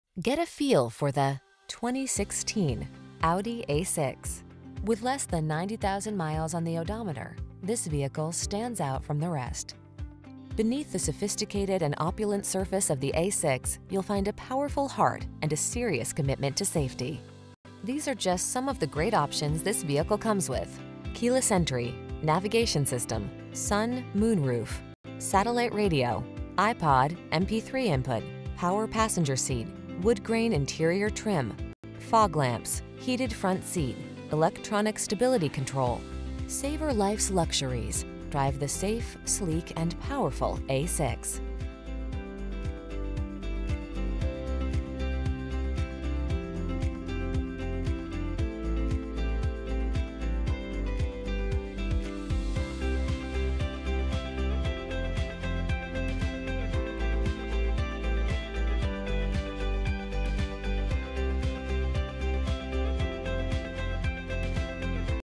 2016 Audi A6 Marietta, Powers Park, Fair Oaks, East Cobb, Sandy Plains 5350
Tornado Gray Metallic Used 2016 Audi A6 2.0T Premium Plus available in Marietta GA at Malones Automotive. Servicing the Marietta, Powers Park, Fair Oaks, East Cobb, Sandy Plains, GA area.

Malones Automotive
1827 Lower Roswell Rd

MSRP $56,800... Includes $8,000 in Options!!..